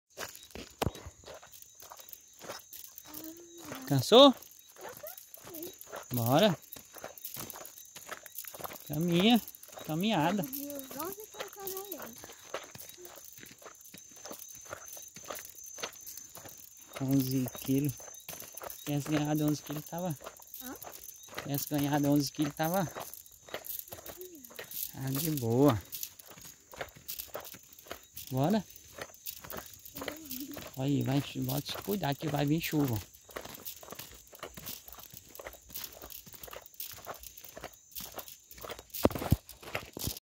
Faz. Acauã-16/04/22 - Eu e Maria Eduarda - Fomos olhar o gado e o cavalo Pé de Pano.
Faz. Acauã 16.04.22 - Eu e Maria Eduarda. Fomos pegar o cavalo Pé de Pano.